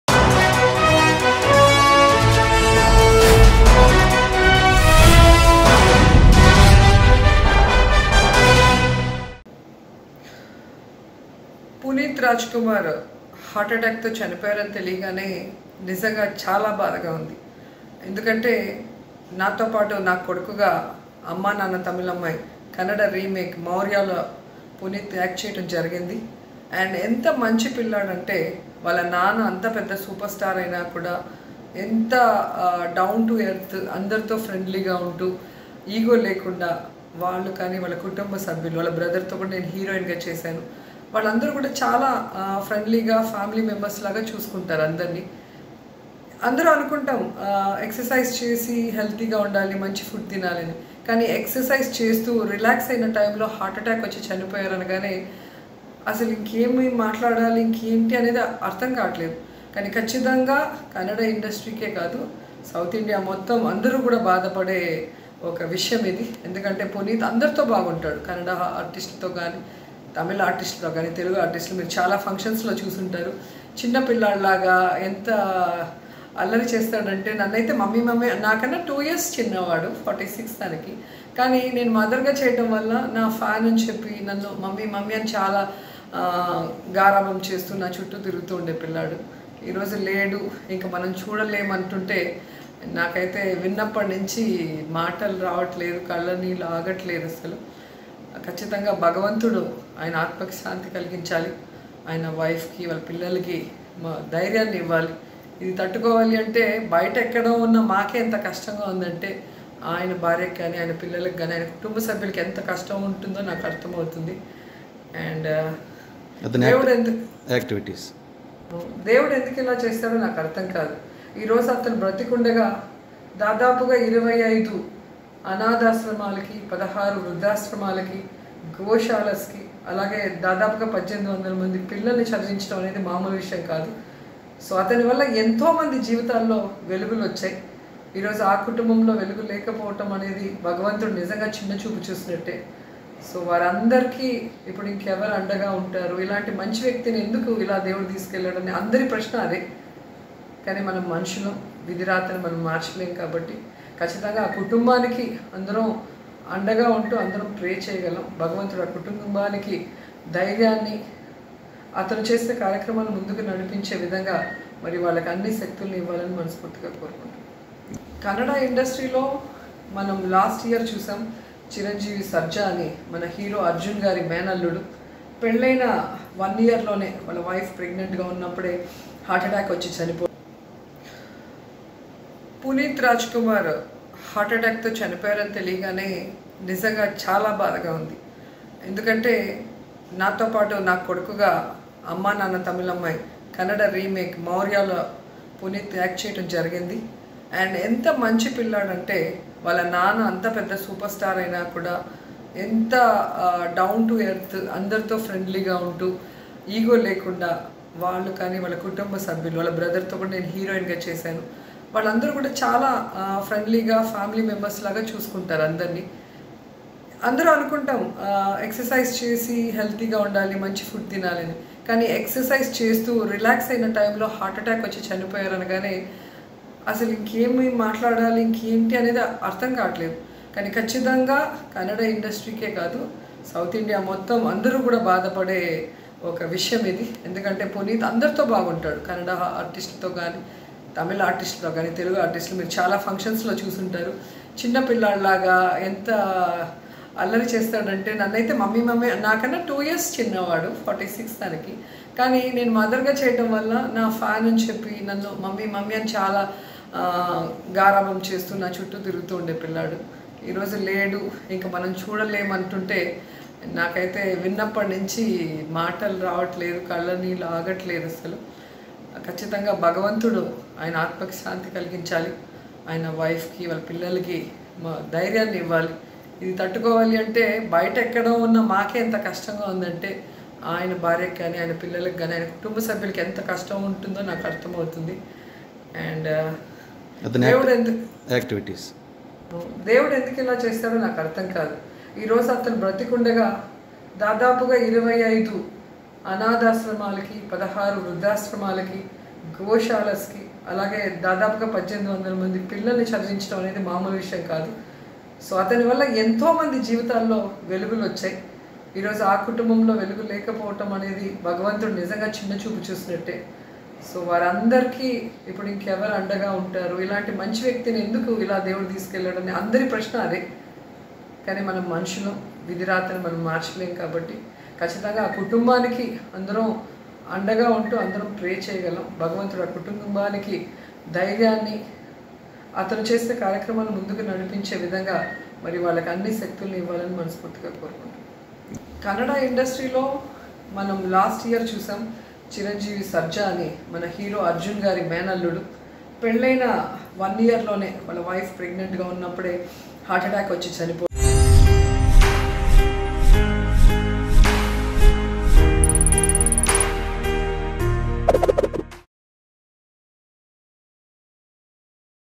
YCP MLA Roja Emotional Words About Puneeth Rajkumar Demise | RIPPuneethRajKumar | Mango News
YCP MLA Roja Emotional Words About Puneeth Rajkumar Demise.